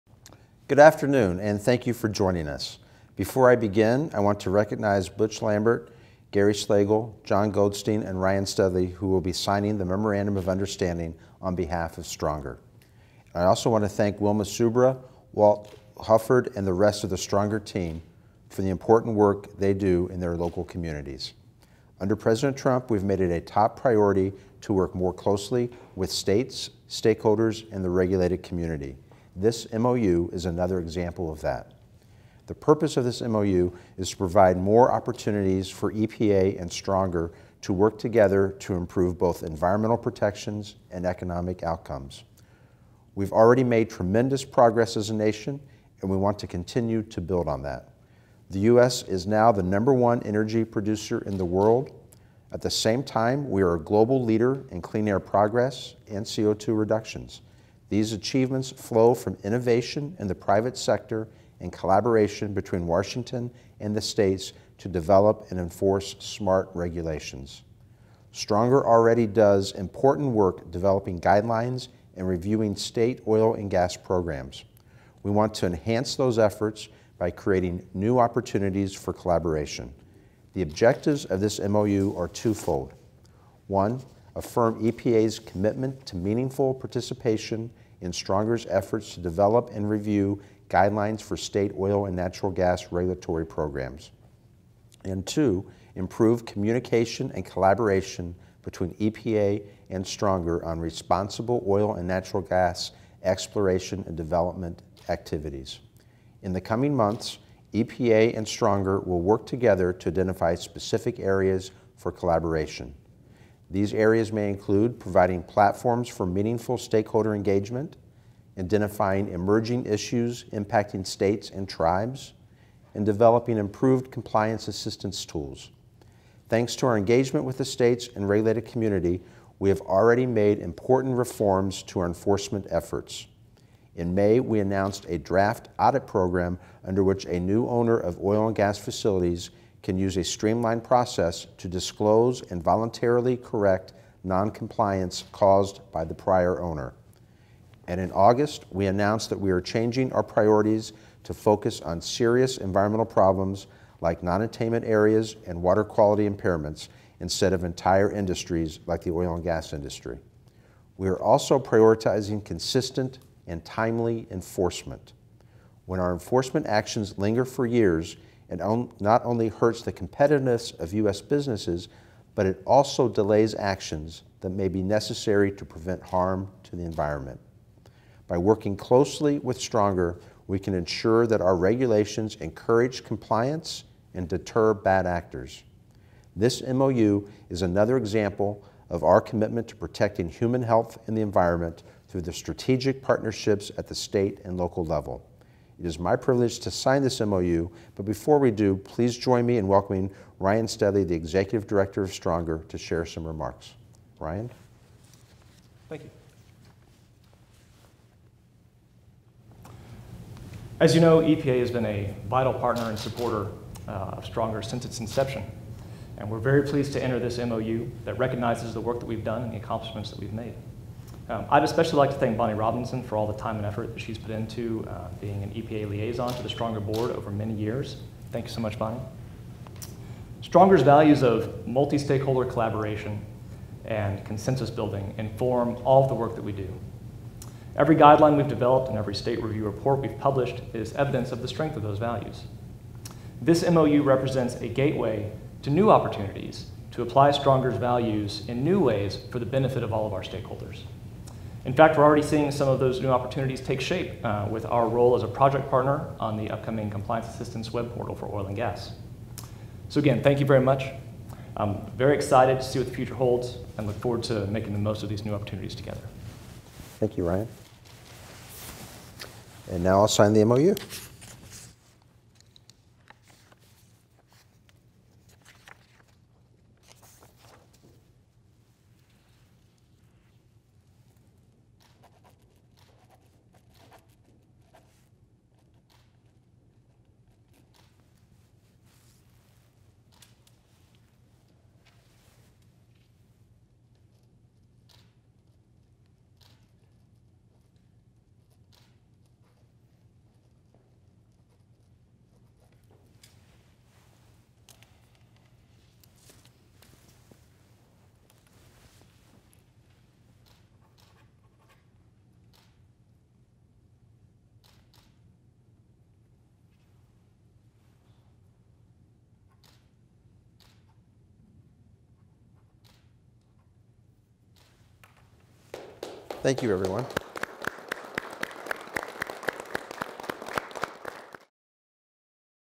EPA Signs MOU with the State Review of Oil and Natural Gas Environmental Regulations
NOTE: This video is being captioned.

The U.S. Environmental Protection Agency entered into a Memorandum of Understanding (MOU) with the non-profit, multi-stakeholder, educational organization known as STRONGER, which is an acronym for State Review of Oil and Natural Gas Environmental Regulations. While STRONGER will continue to develop guidelines and conduct reviews of state oil and natural gas programs, the MOU will provide the parties with greater opportunities for collaboration that are in the mutual interest of both STRONGER and EPA.